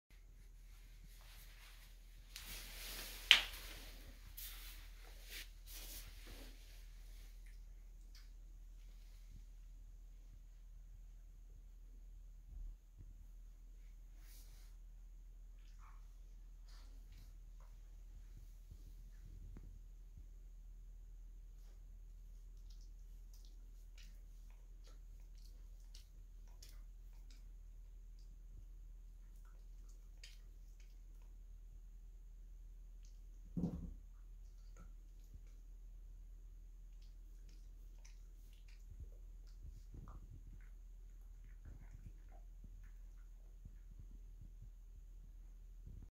Super Cute Puppy's First Time Seeing Himself In A Mirror - Funny Licking & Staring At Reflection
This is our adorable German shepherd and husky mix puppy seeing himself in the mirror for the first time.